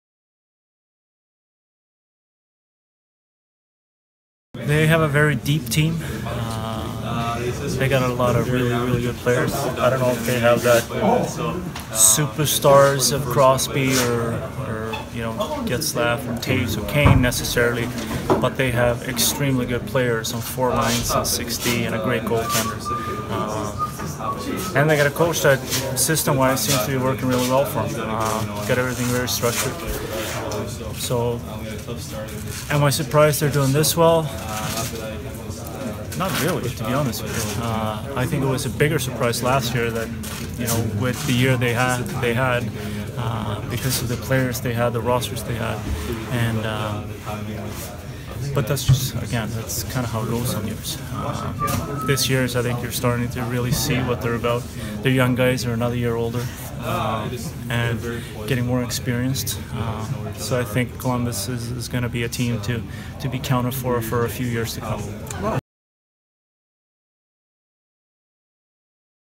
Niklas Kronwall on what makes Columbus Blue Jackets so good
The Red Wings play Saturday in Columbus.